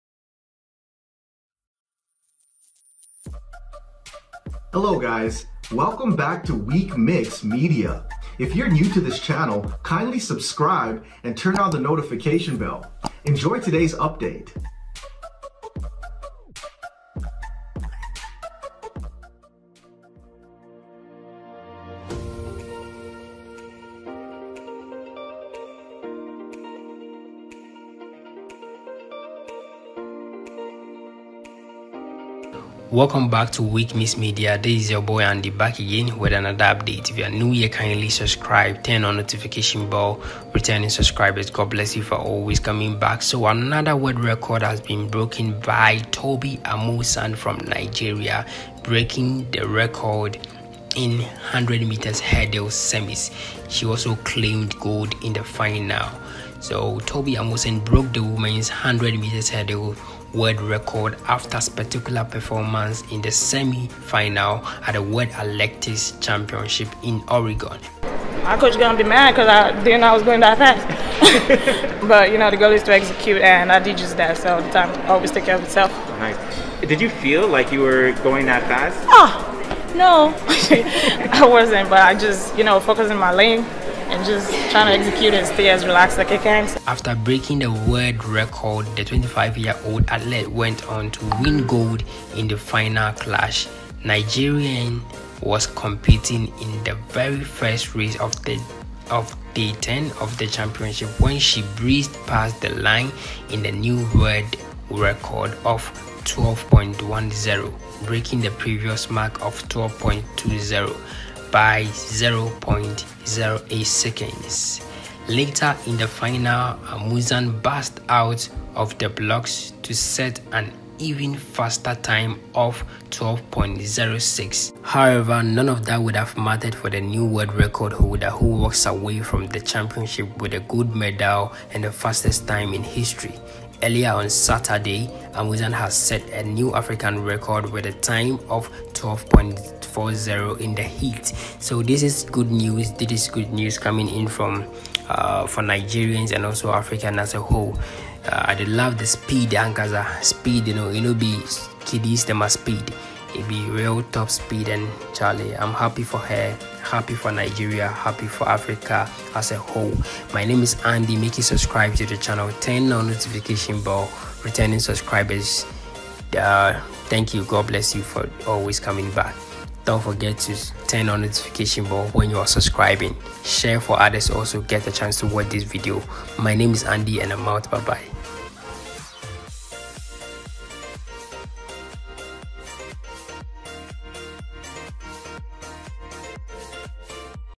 Chaeta like speed by Tobi Amusan as she breaks and set world record with 12.06s in the 100m Hurdles
Nigerian Tobi Amusan set new world record at the World Athletics Championships in Oregon